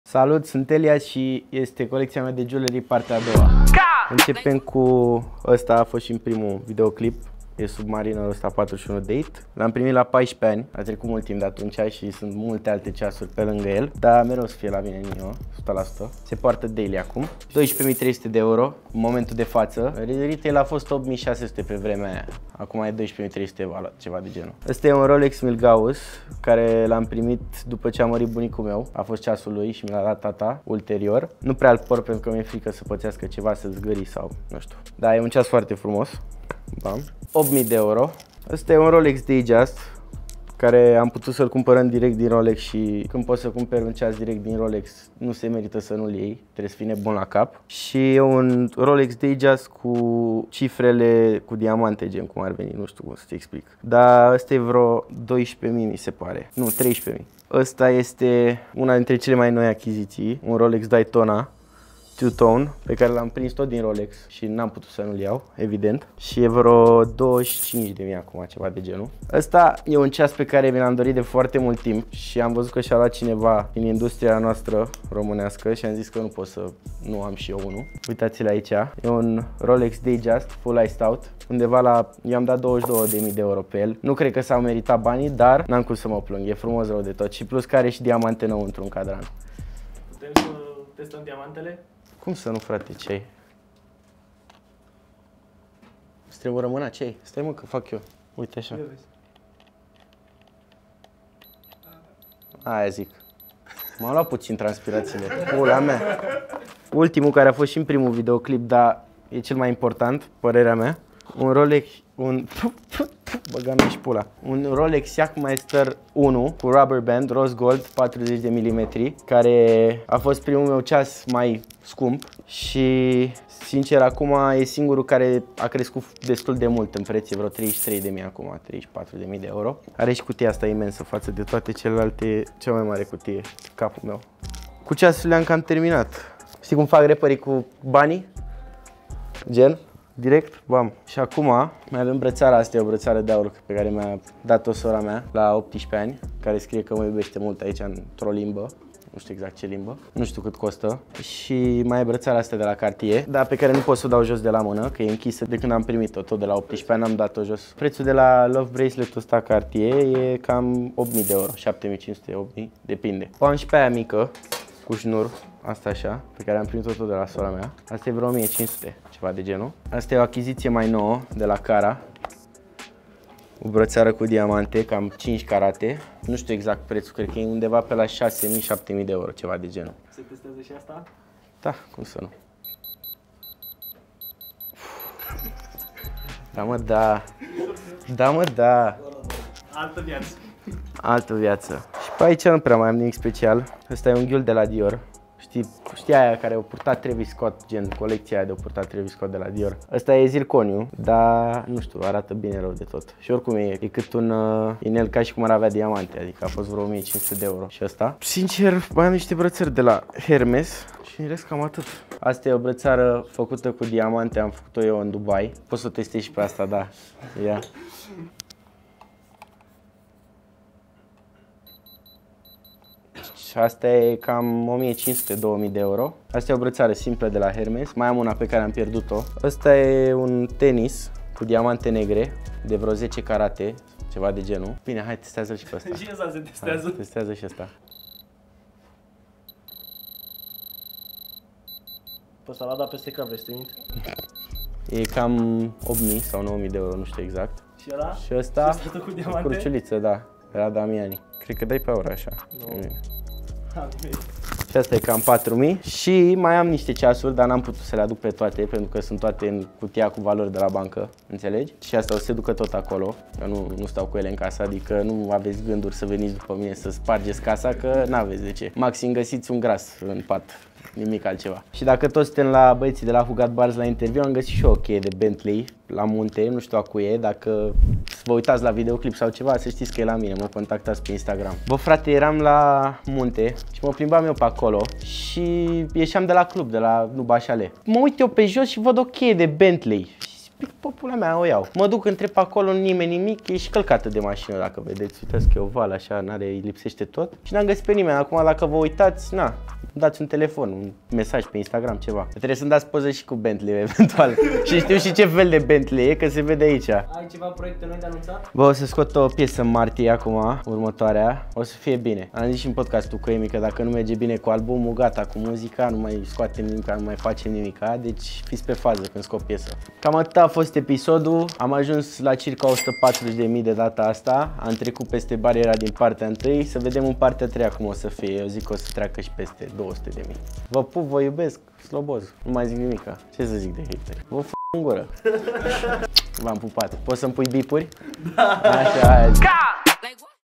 ELIAS PREZINTA COLECTIA DE BIJUTERII PARTEA 2 (150.000€) - WHOGOTBARS
Elias isi prezinta colectia de bijuterii, partea a 2-a (150.000€) + diamond tester

WHOGOTBARS Entertainment 2023.